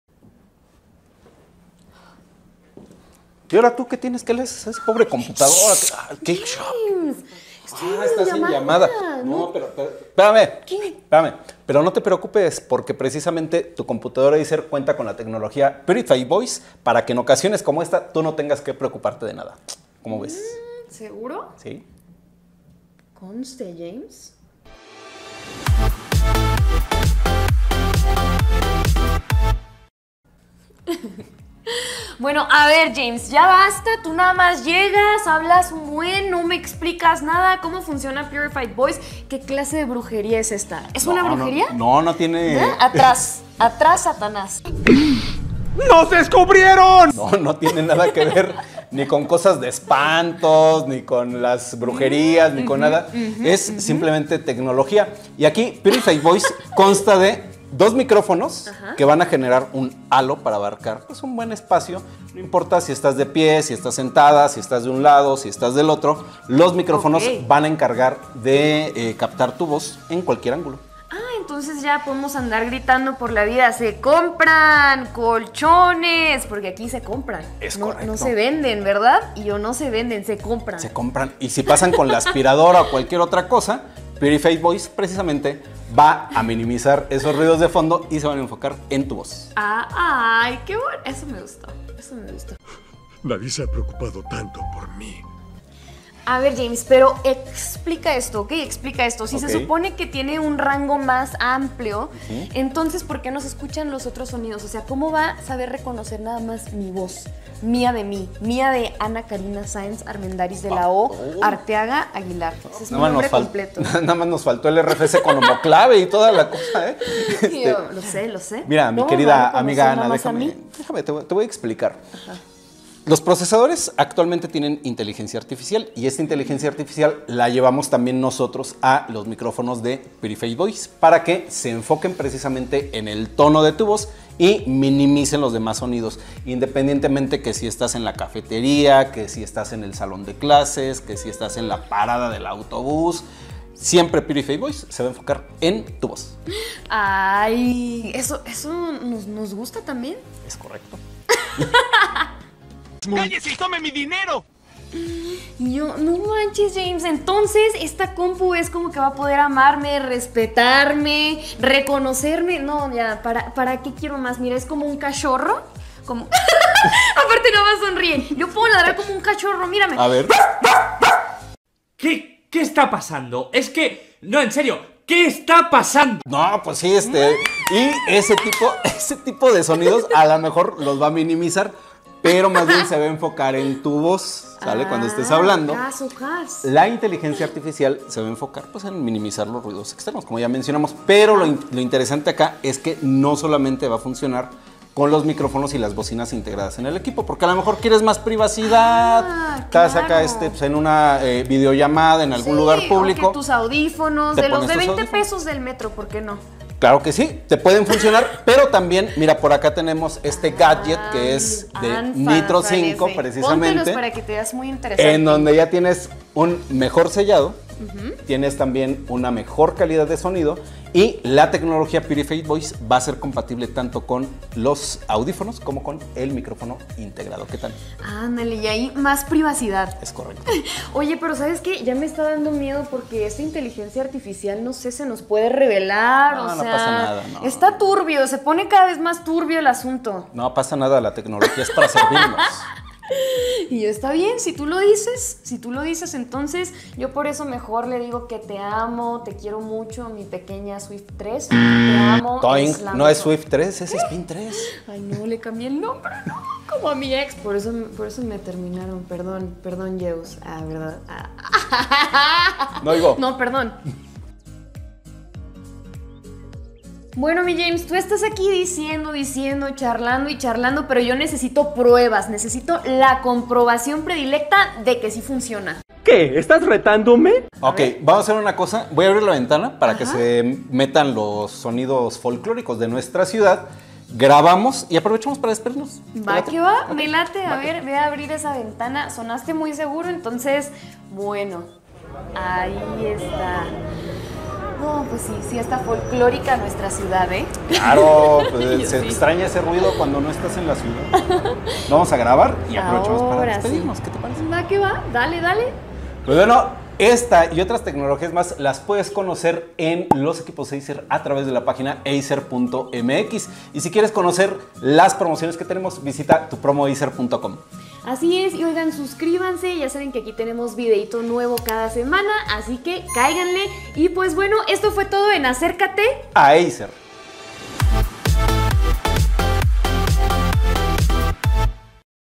Cómo tener buen audio en videollamadas (Purified Voice)
Ten videollamadas con un audio perfecto gracias a la tecnología Purified Voice incorporada en las laptops Acer. 😱💻
No tienes que hacer nada para acivarlo, sólo disfrutar del futuro.😎